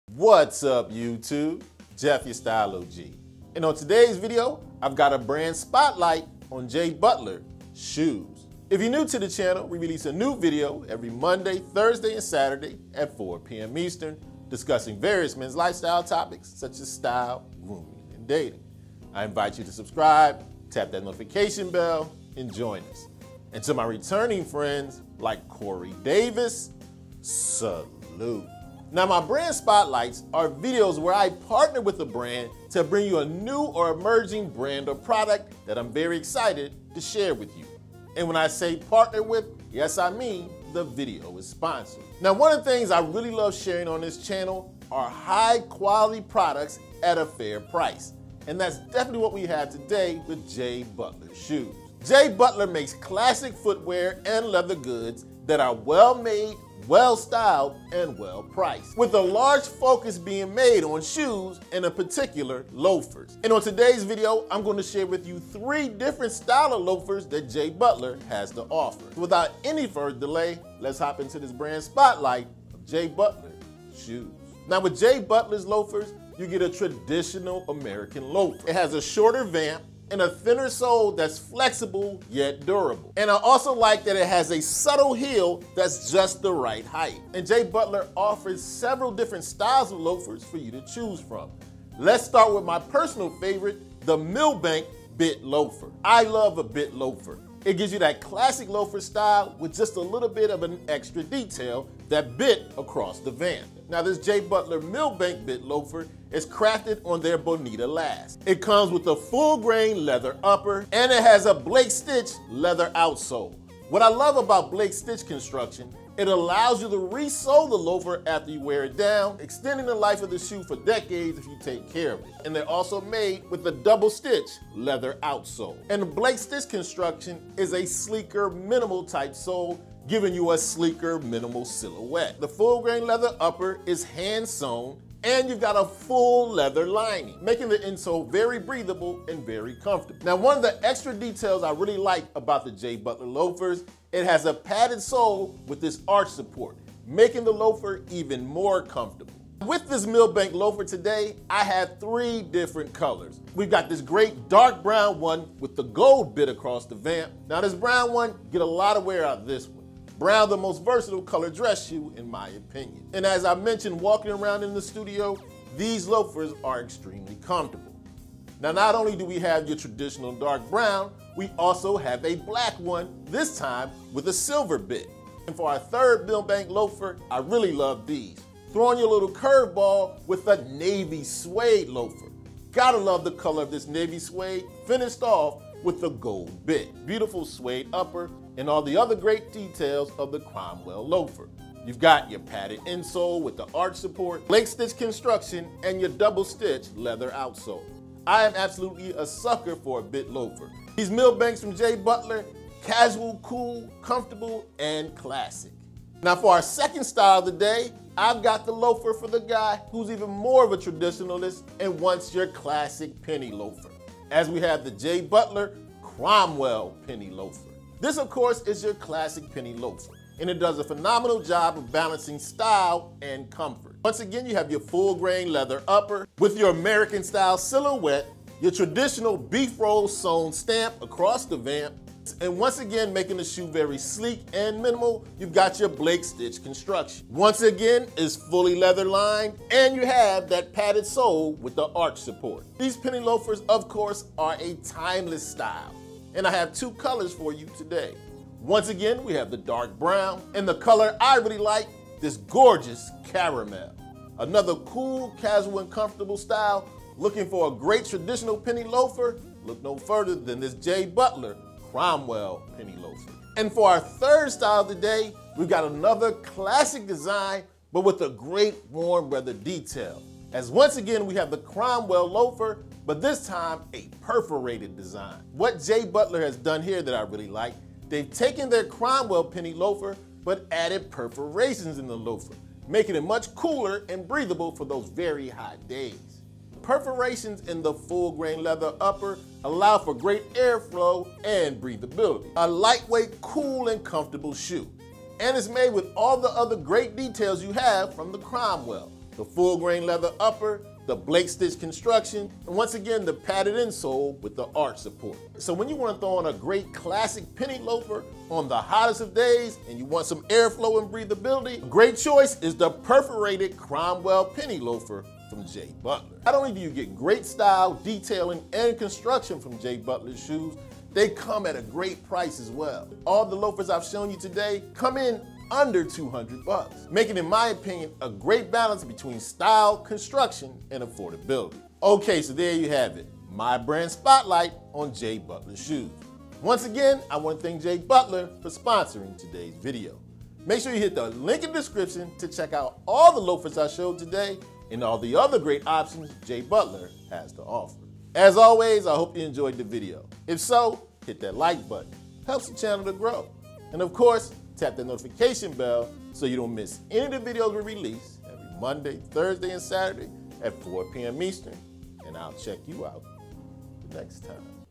Jay Butler Shoes Brand Spotlight
In this video, Jeff from The Style O.G. shines a brand spotlight on Jay Butler Shoes. He talks about the style, make. comfort, and affordability of the loafers.